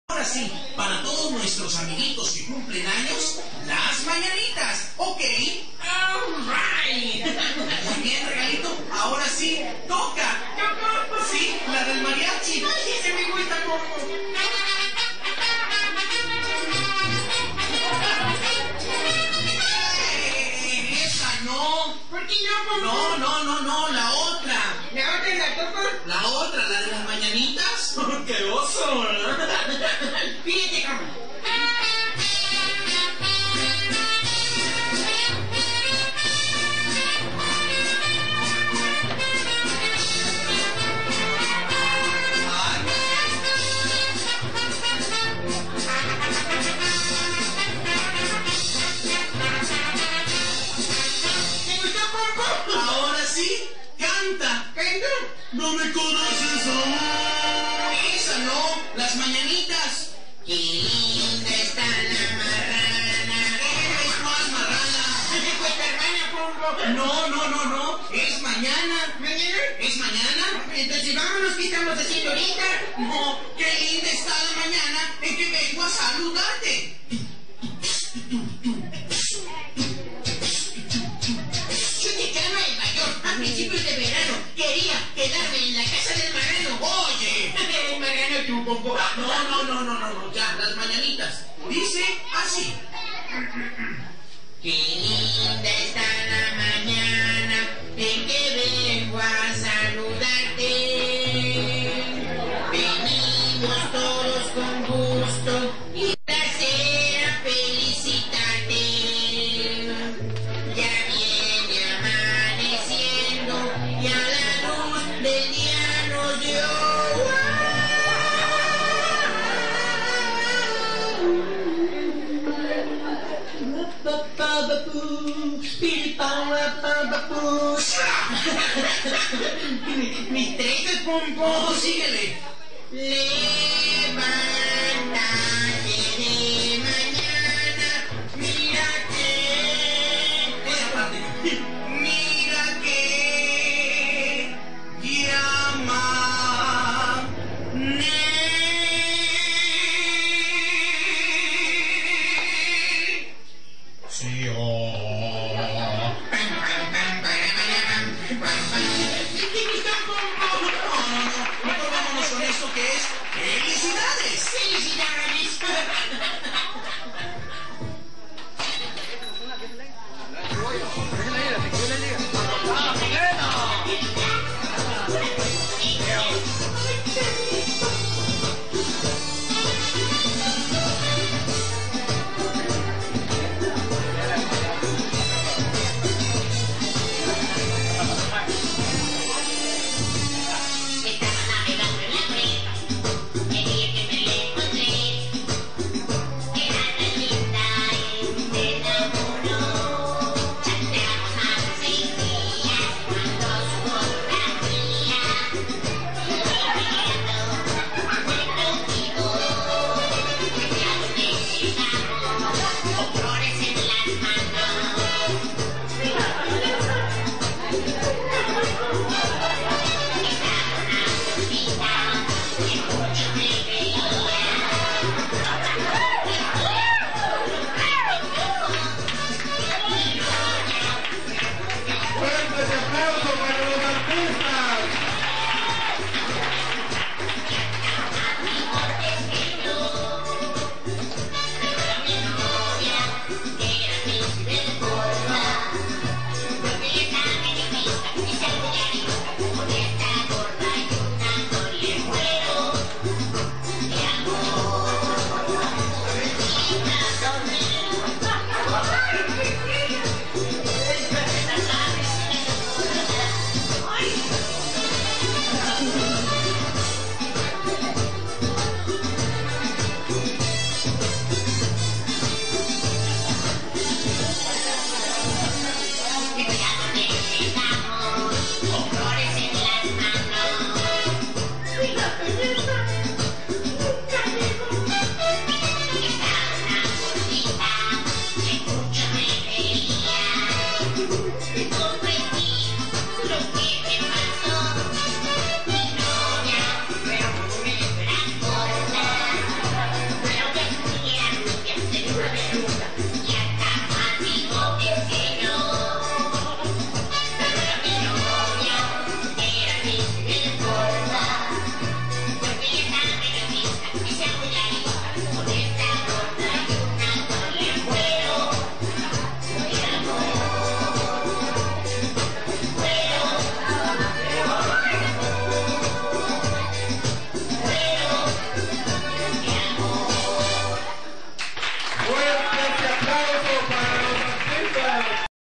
Velada Artistica 2009/ La Cyber gorda
E aqui una muestra de lo que fue la primer velada artistica correspondiente a este año en Asuncion Amatepe,quiero comunicar que no podre subir todos los videos de esta velada ya que son muchos y por consecuente solo podre subir los que mas le han gustado al publico espectador de esta velada artistica.
Si te gustan los videos te recuerdos que puedes disfrutarlos en vivo y en directo el Sabado 8 de agosto de 2009 en Asuncion Amatepe, Rosario de la Paz, La Paz, El Salvador C.A. como una antesala  a las fiestas patronales de este bello canton que poco a poco lo estamos dando a aconocer al mundo Atte. Erick Preza.
Actuaron para ustedes: Roberto Carlos Ramos, Erick Prexza y Luis Angel Ramos.